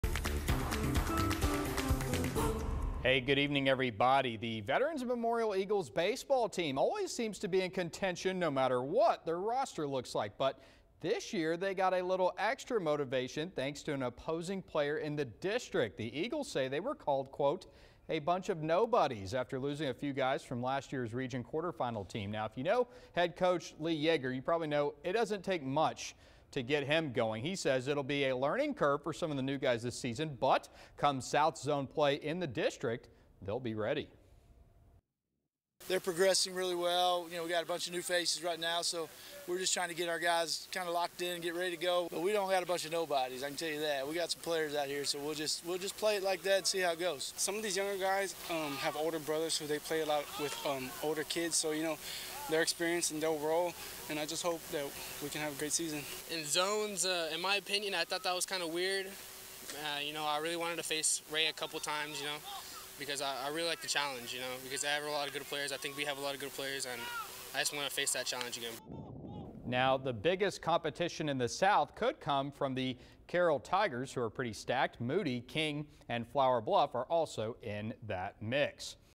Veterans Memorial baseball: a "bunch of nobodies?"
The Eagles say they got a little extra motivation after losing a few key players from last year's region quarterfinal team.